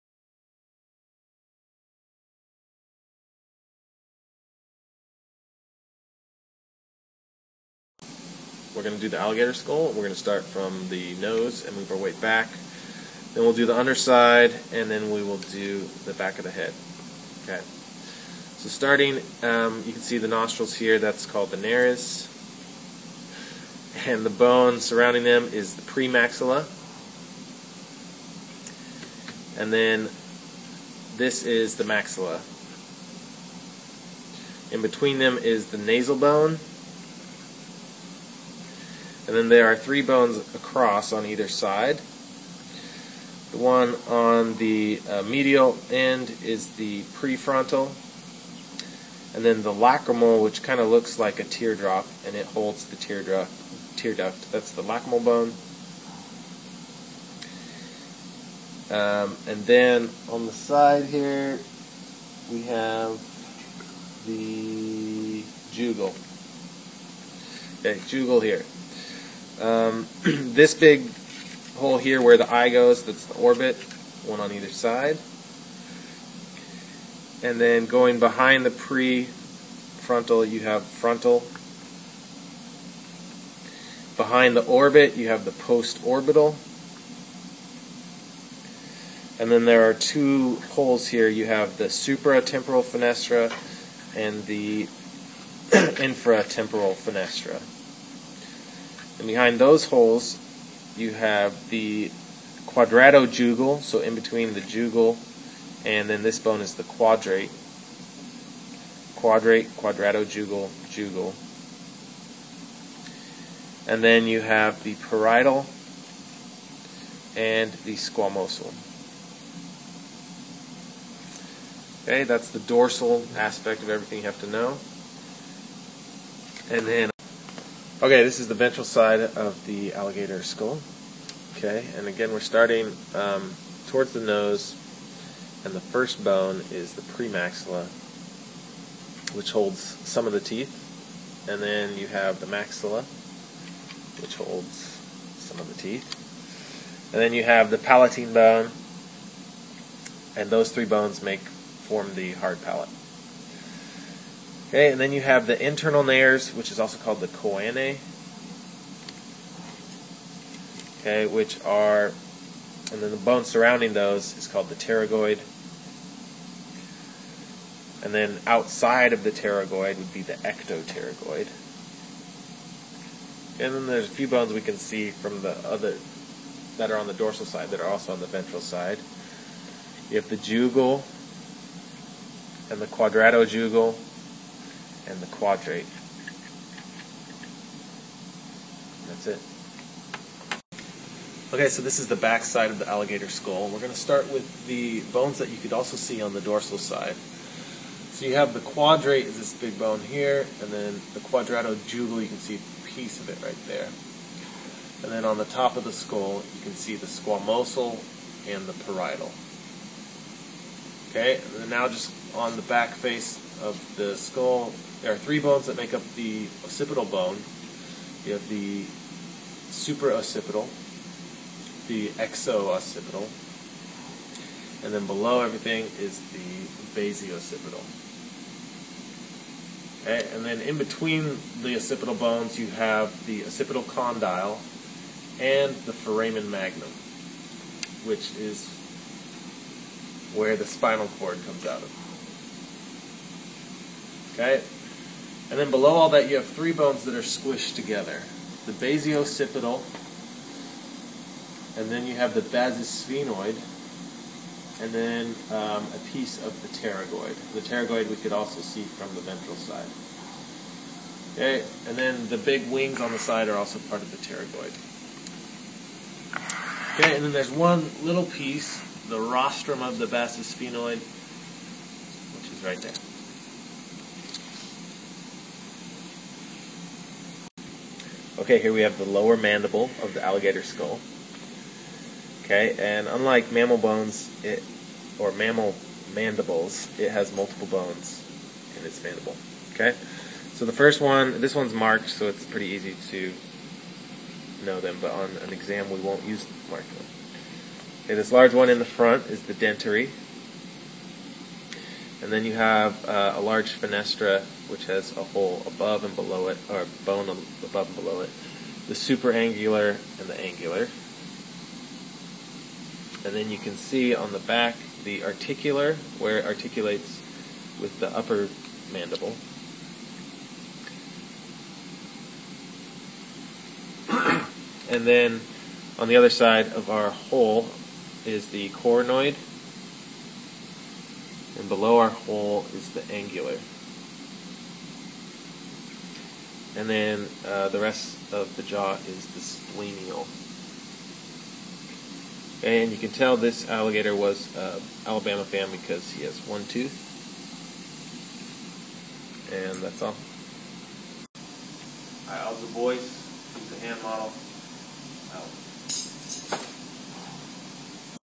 Anatomy of the Alligator Skull
A video tutorial of the anatomy of the alligator skull for comparative anatomy